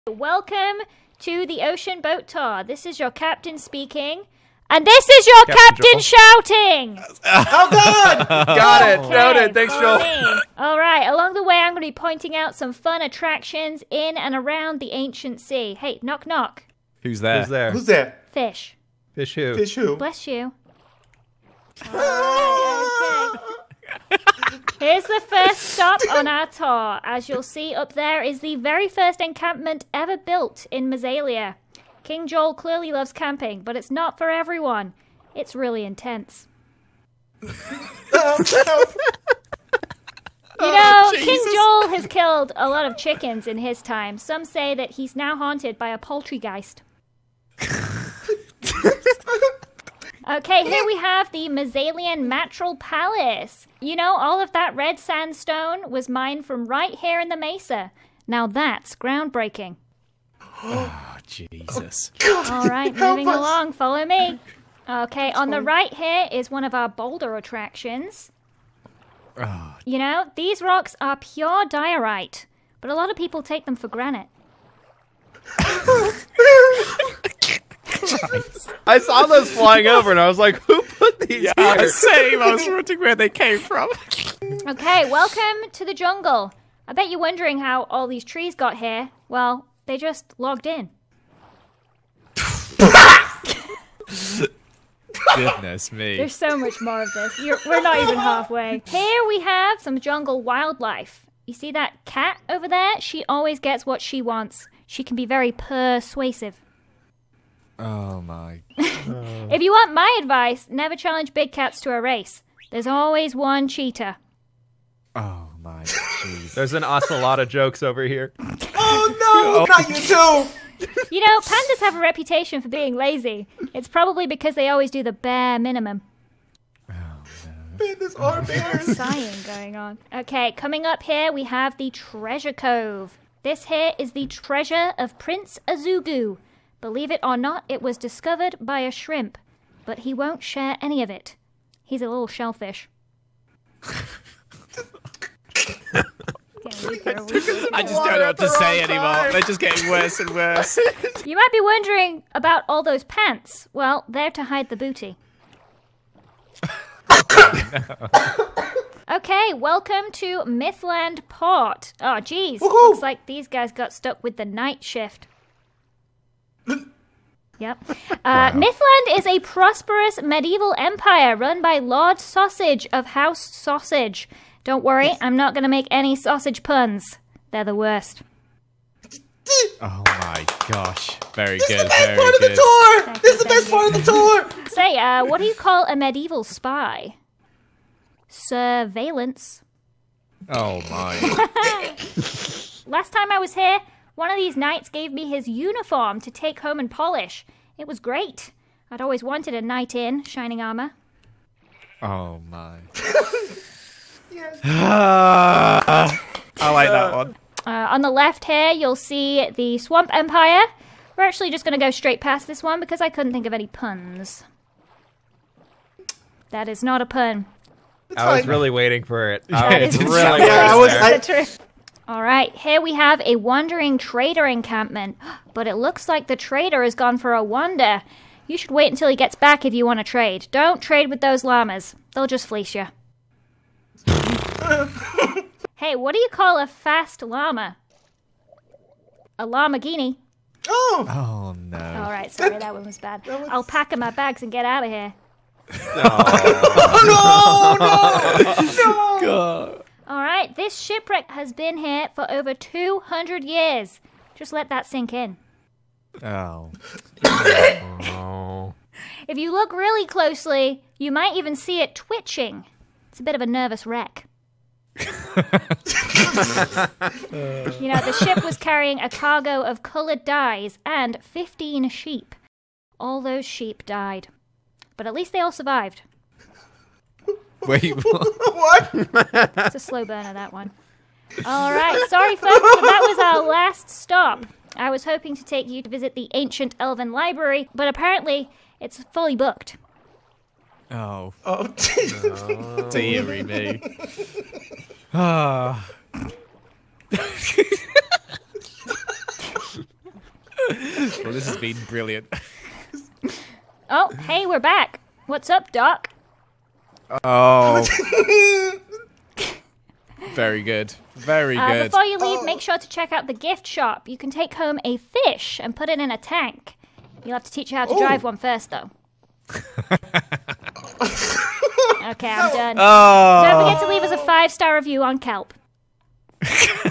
LDShadowLady making cheesy puns for 5 minutes and 27 seconds
Please *boop* the like button if you enjoy the video! :)